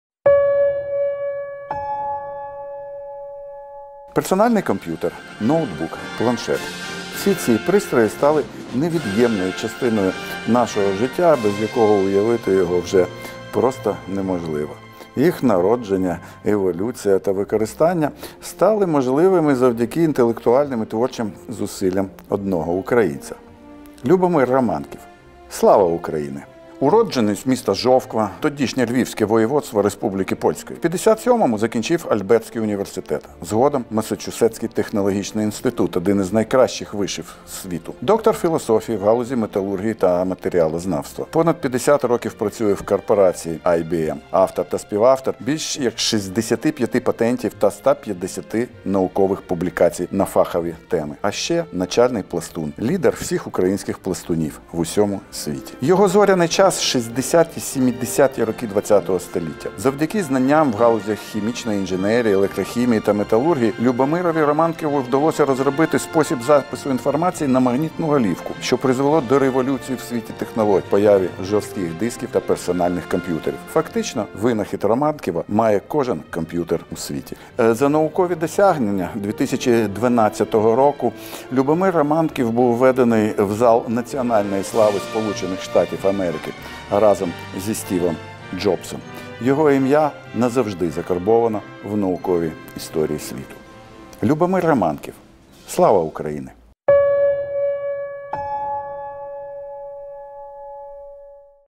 Любомир Романків | Слава України №6
Любомир Романків. Наукові відкриття цього видатного українця назавжди змінили історію світу, подарувавши йому персональний комп‘ютер, без якого сьогодні немислимо існування людей на Землі. У 1962 році Любомир винайшов магнітно-опірні мікроголовки для запису інформації, що зробили переворот у комп’ютерній індустрії. Саме мікроголовки Романківа проклали шлях до появи жорстких дисків. Першим винахід українця оцінив Стів Возняк - співзасновник компанії Apple. Саме він закупив у ІВМ перші жорсткі диски та разом зі Стівом Джобсом незабаром випустив перший настільний комп’ютер Apple-1.